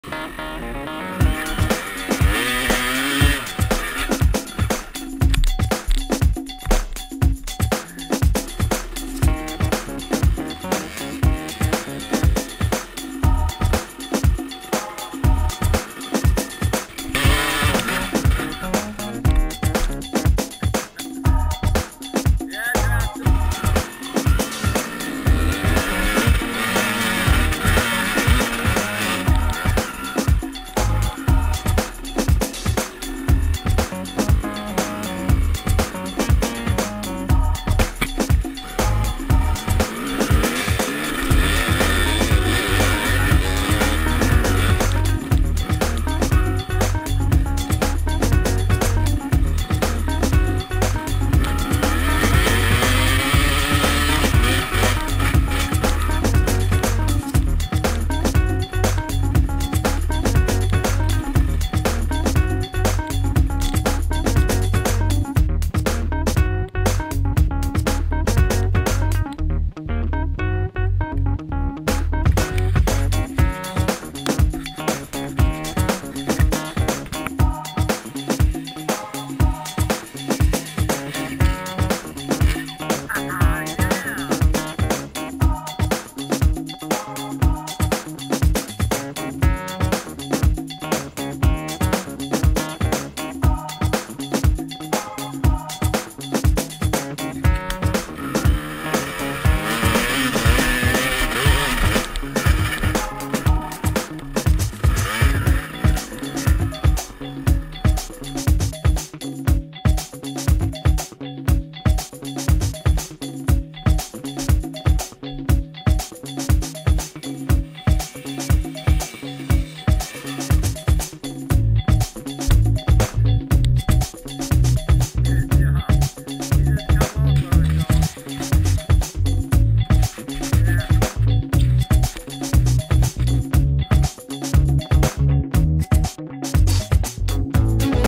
Levi @ Perry State Forest.wmv
Some of Levi's first attemps at Sand paper and Killer at Perry State forest